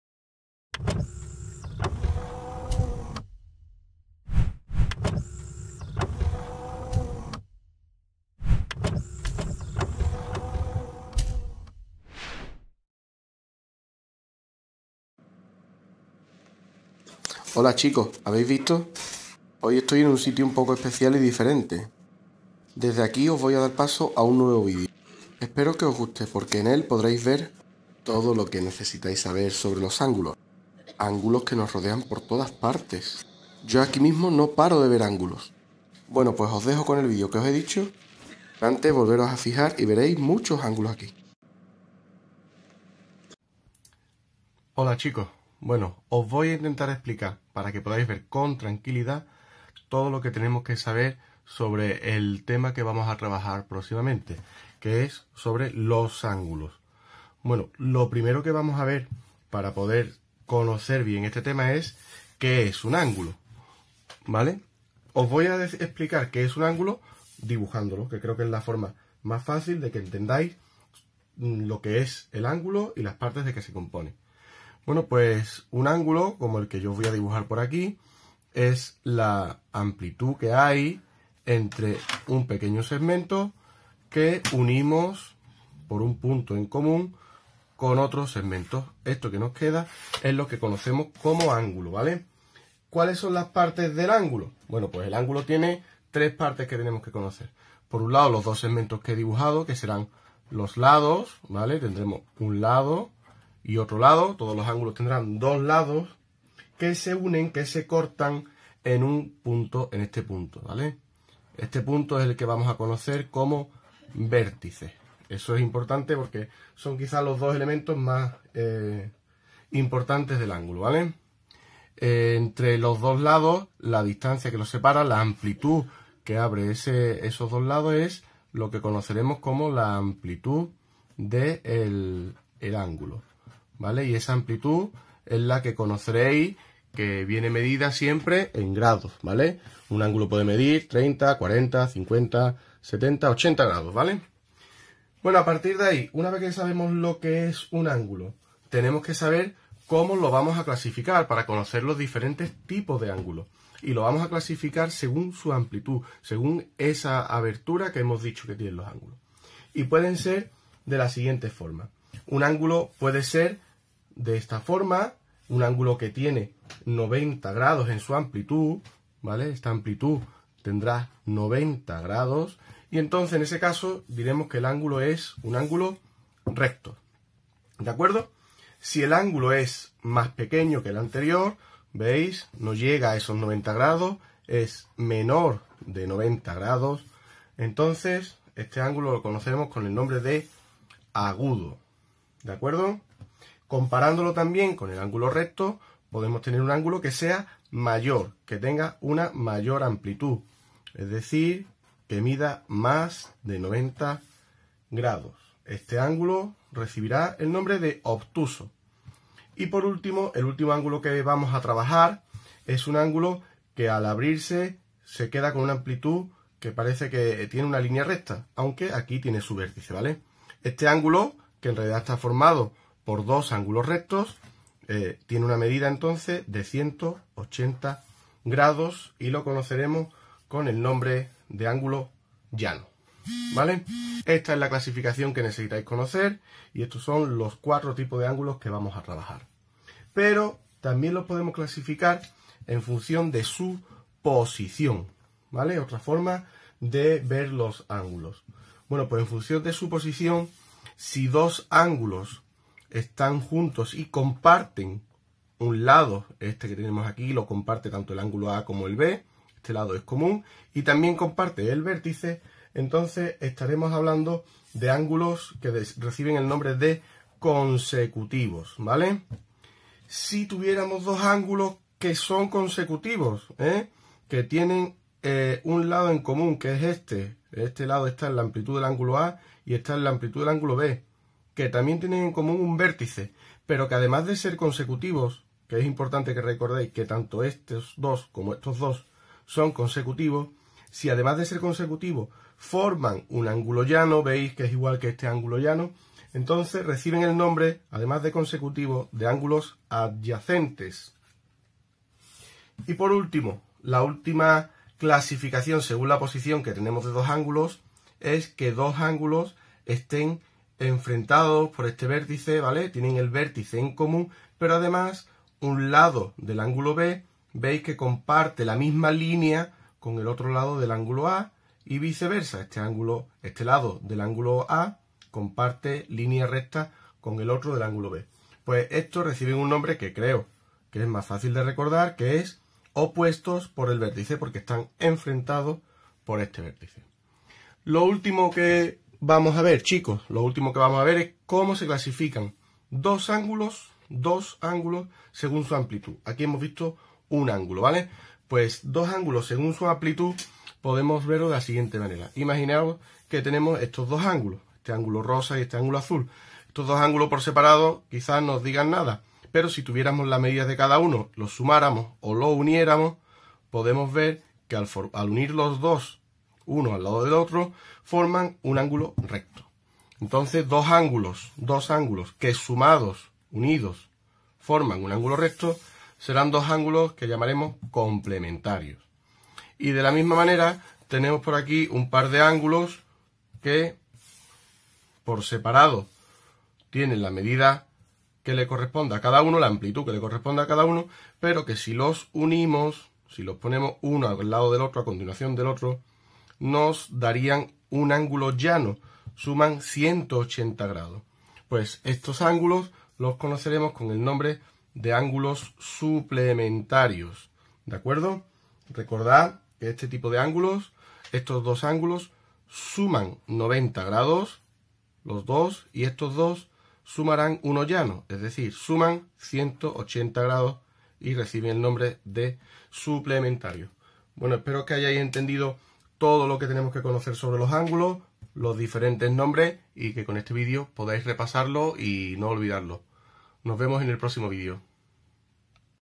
Conocemos LOS ÁNGULOS
Vídeo con la explicación de qué son Los Ángulos, características y tipos ... El primero de la serie con la que intento DAR LA VUELTA A MI CLASE!!!